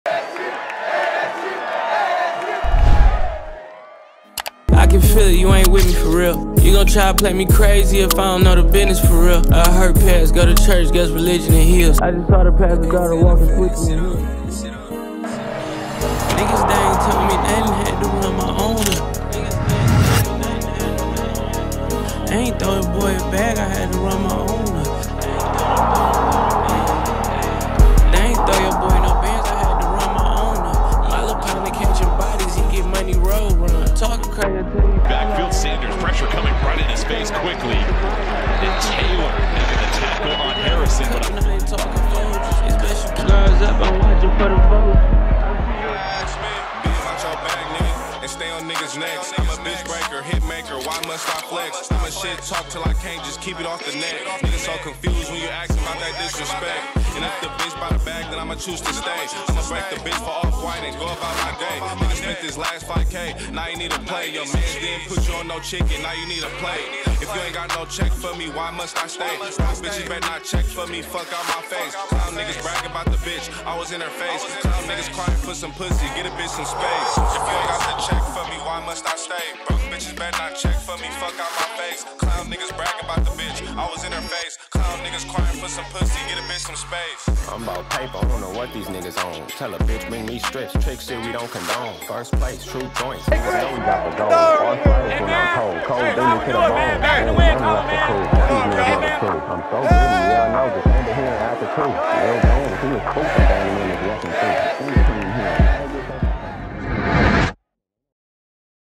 Alphonso Taylor Jr, 2023 Mixtape
Meet Alphonso Taylor Jr, the epitome of a safety prototype! Standing at an impressive 6'3" and weighing 215 pounds, this dynamic safety possesses a super-long frame and exceptional open-field tackling abilities.

Projected to shine as a strong safety or a versatile nickel corner, Taylor is set to make a significant impact in both run support and coverage against slot receivers. His prowess in man-to-man coverage with tight ends is truly remarkable.

In my opinion, Taylor has all the potential to secure a spot on a 53-man roster. Join us as we delve into the skills and potential that make Alphonso Taylor Jr a defensive force to be reckoned with! 🌟🔒🏆